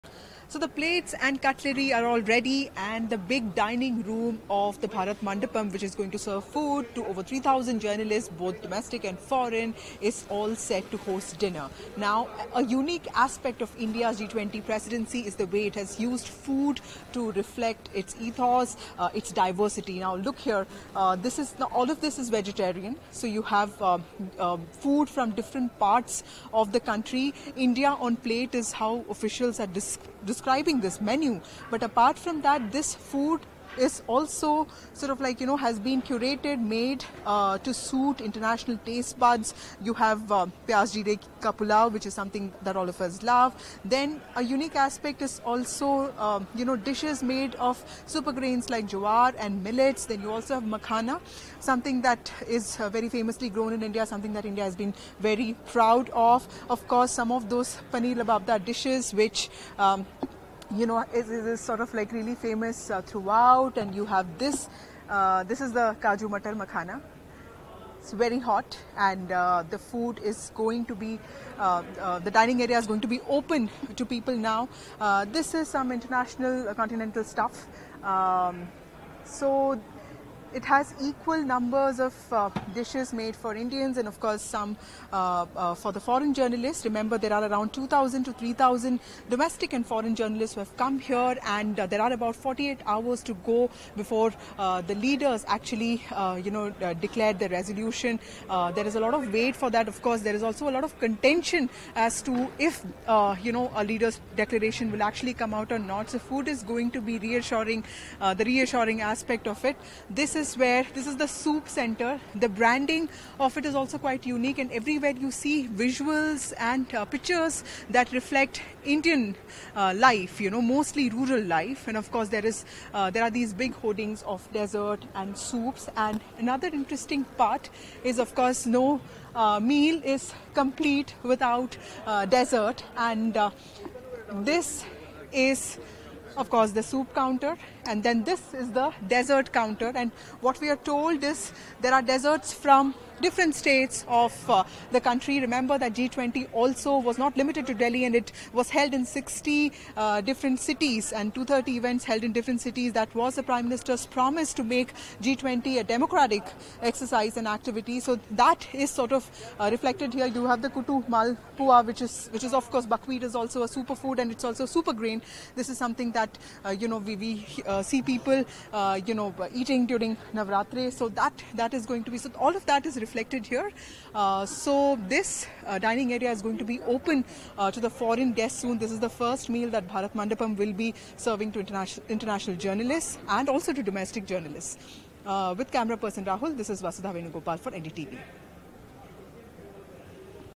G20 Summit | Salad, Superfoods Part Of Elaborate Vegetarian Spread For G20 Journalists
The big dining room at the Bharat Mandapam - the G20 venue in Delhi - is all set to host over 3,000 foreign and Indian journalists for dinner. How food has been used to reflect the country's ethos and diversity is a unique aspect of India's G20 Presidency. NDTV's Vasudha Venugopal brings all the details of food being served to journalists.